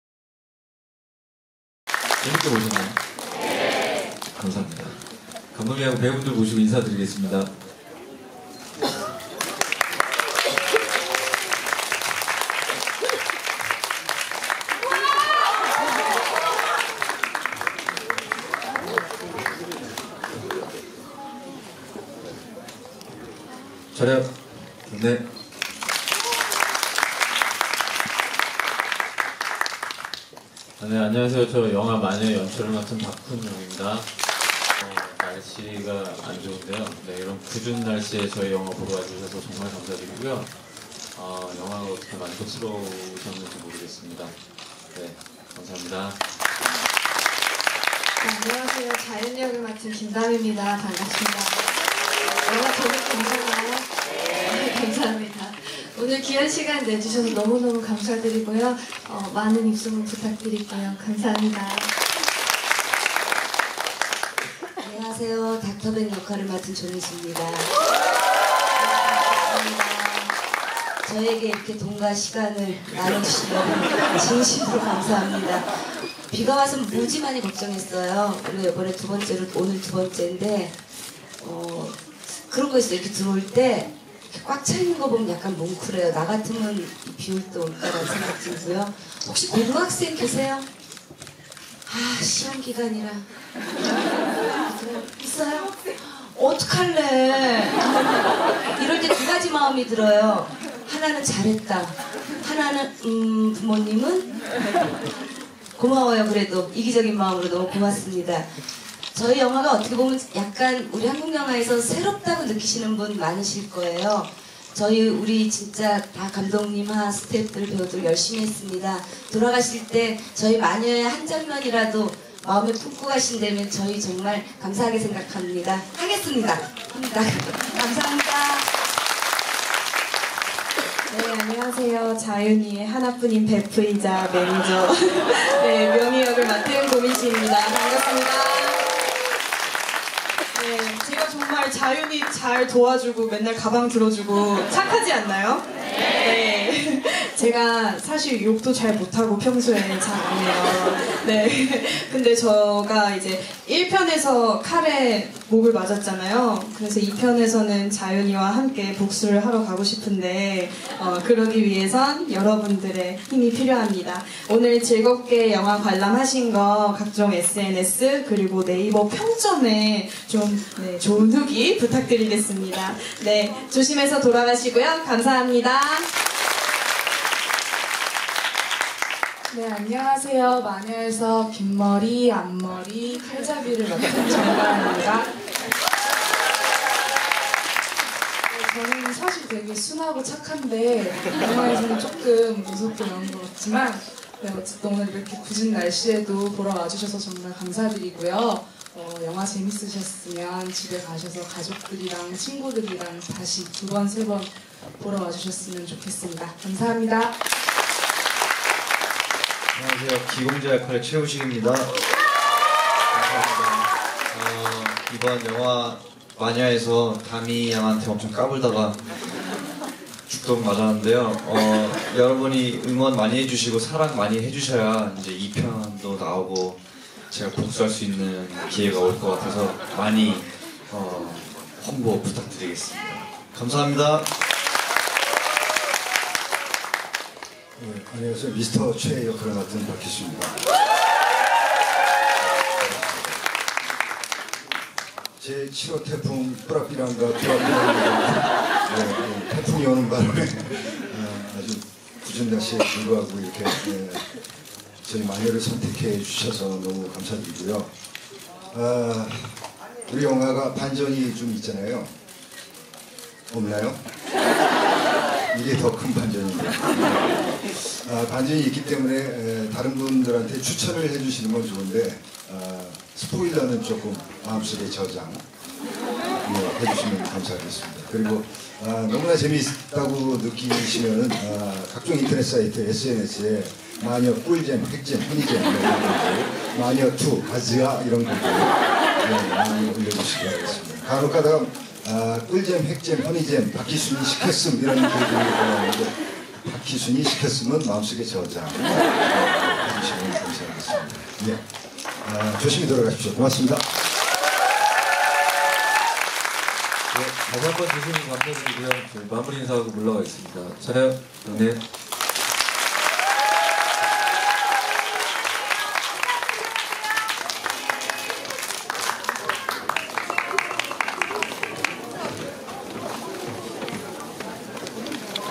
[뉴스인스타] 180701 영화 '마녀' 김다미 최우식 조민수 박희순 무대인사 직캠 영상
7월1일 오후 서울시 용산구 CGV 아이파크몰에서 영화 '마녀' 무대인사가 열렸다. 이날 무대인사에는 박훈정 감독, 김다미, 조민수, 고민시, 정다은, 최우식, 박희순이 참석한 직캠 영상